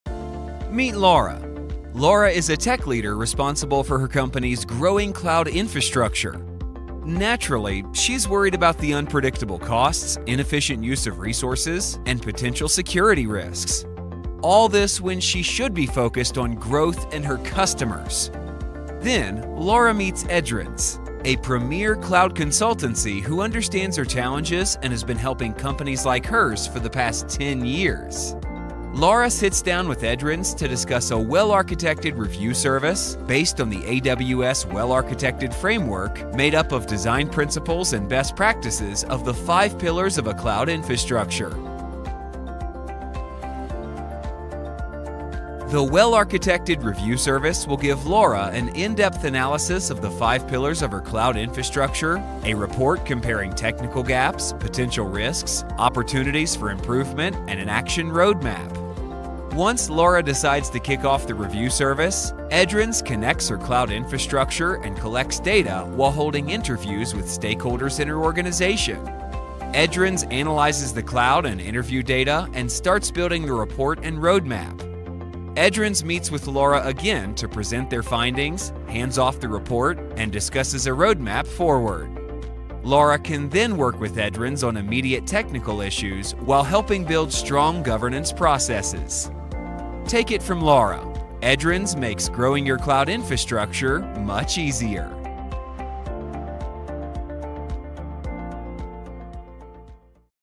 Well Architected Reviews by Edrans
The Well architected review by Edrans is one step beyond just reviewing your technical infrastructure, it also focuses on your organization processes and priorities, giving you a holistic view of your Cloud Organization.